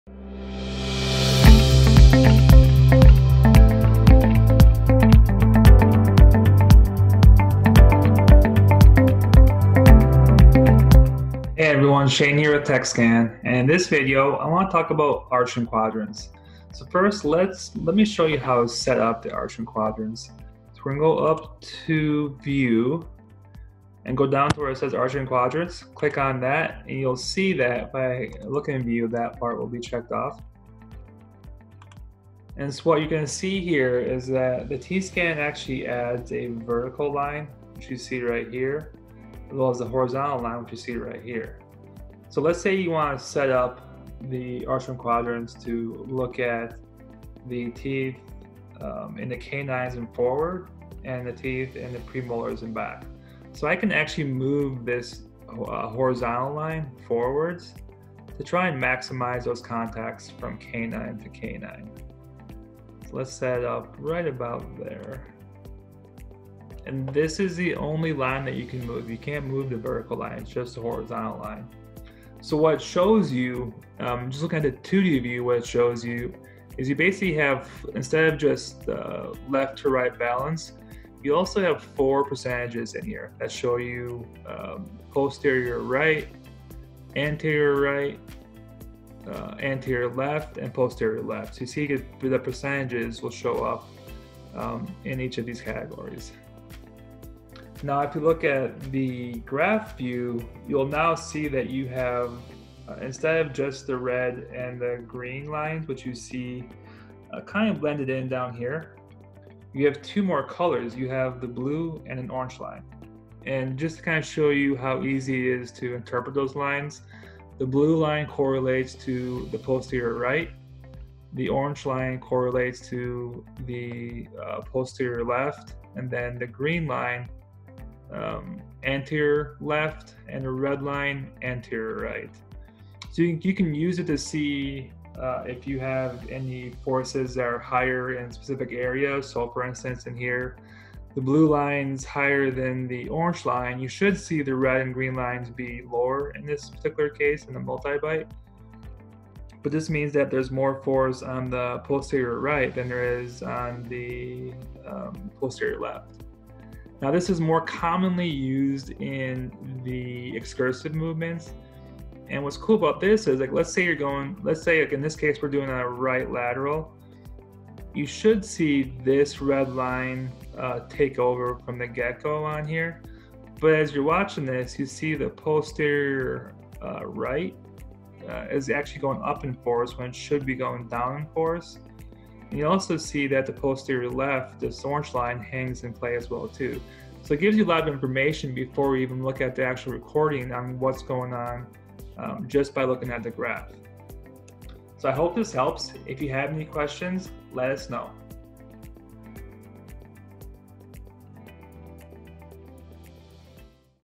Setting up Arch & Quadrants in T-Scan Software
In this video, Shane Varga, Regional T-Scan Sales Manager for Tekscan, demonstrates how dentists can use the Arch & Quadrants feature in T-Scan software to:

- Analyze contacts from canine-to-canine, molar-to-molar, etc.
- How to correlate your timeline to posterior and anterior contacts